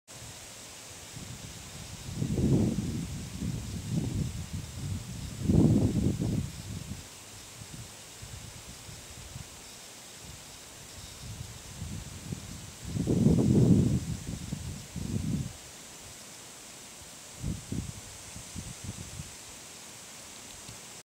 Nevada Buck Moth (Saturniidae: Hemileuca nevadensis) Male on Road
Photographed at the Rydell NWR, Minnesota (06 September 2010).  Thank you to 'Ryan' (@Bugguide.net) for confirming the identification of this specimen!